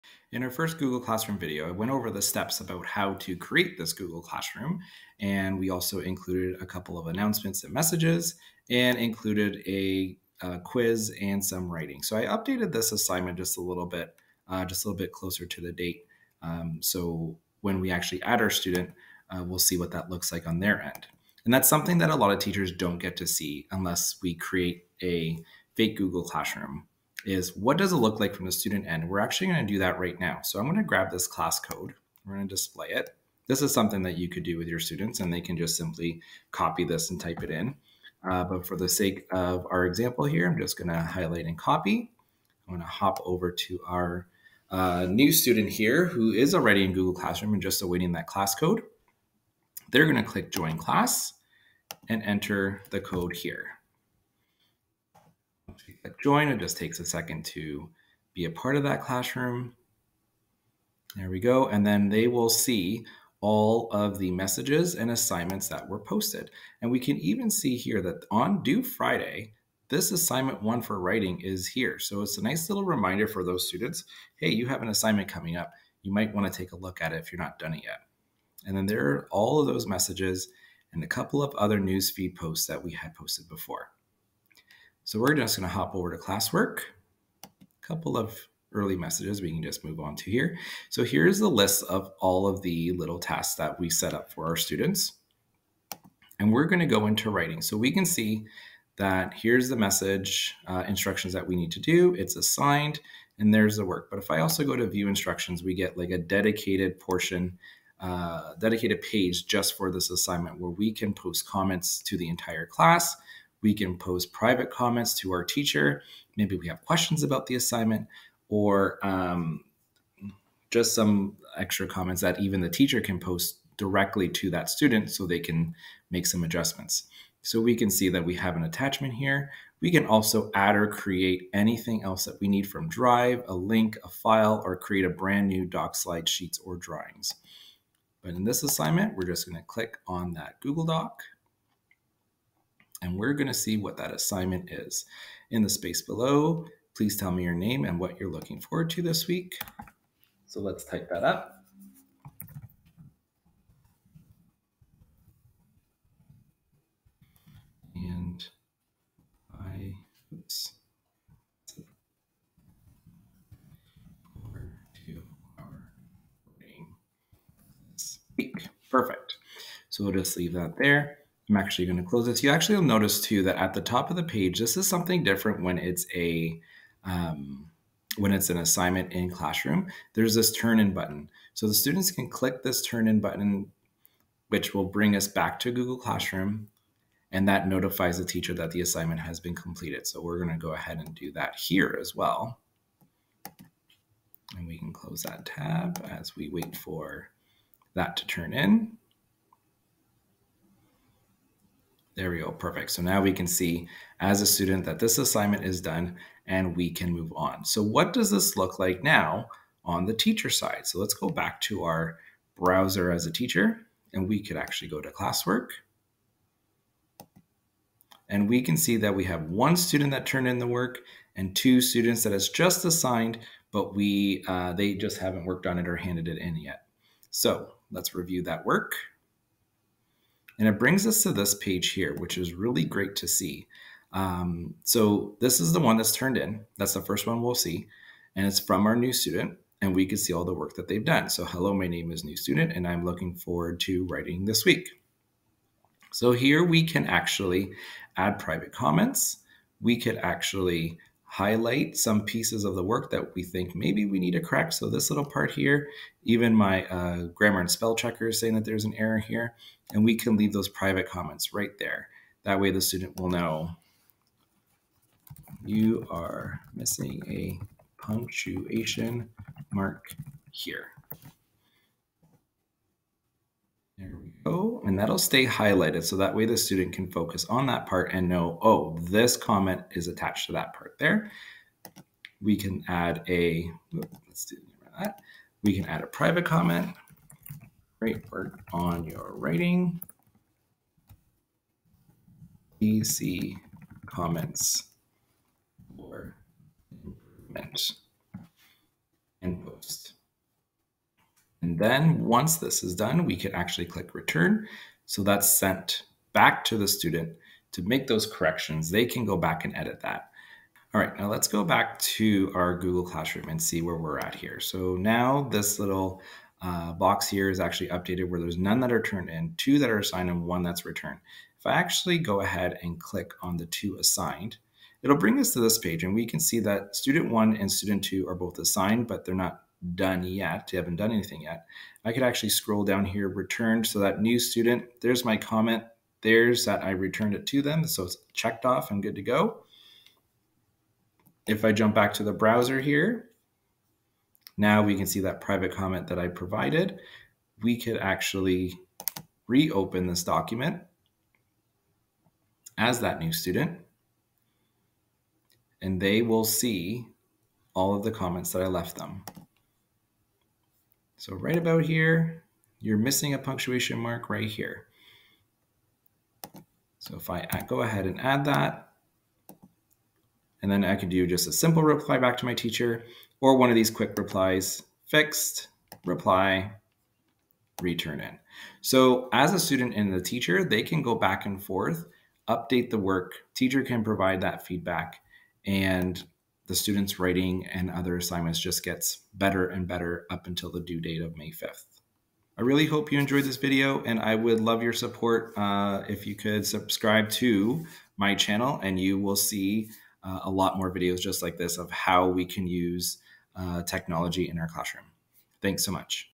Google Classroom: What The Students See!
In this video, we'll take you through a step-by-step guide on how to use Google Classroom as a student. Whether you're new to Google Classroom or just need a refresher, we'll cover everything you need to know to get started. From joining a class to submitting assignments, we've got you covered. Join us as we explore the many features of Google Classroom and show you how to get the most out of this powerful online learning platform.